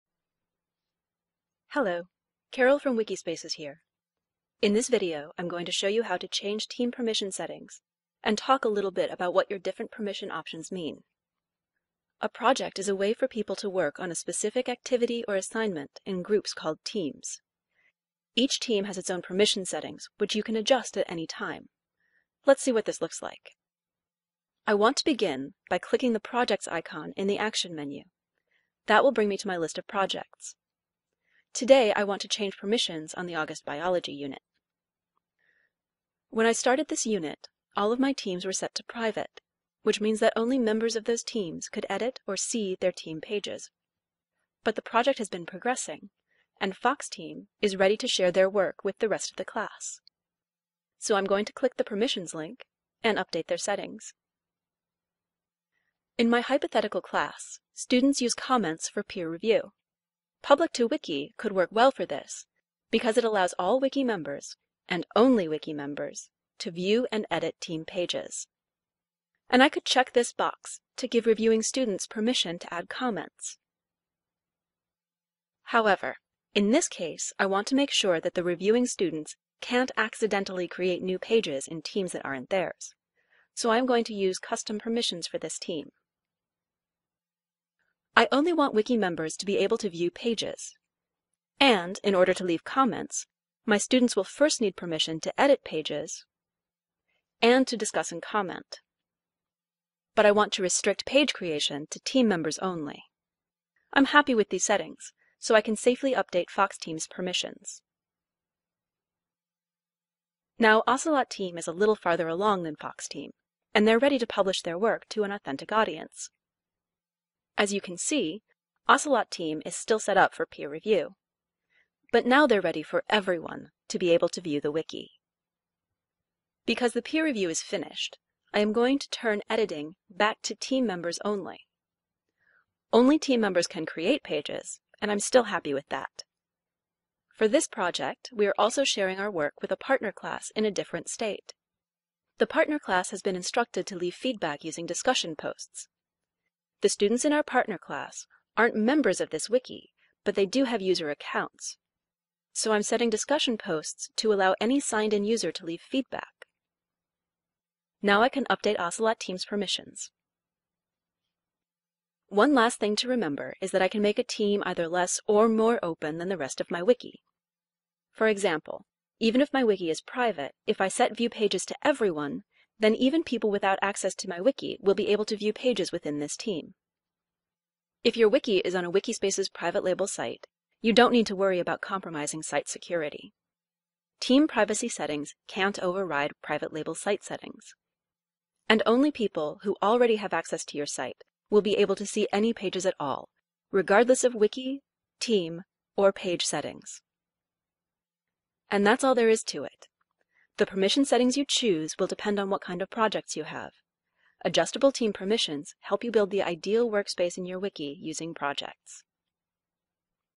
Projects: Team Permissions
Set individual permissions for all the teams in a Project.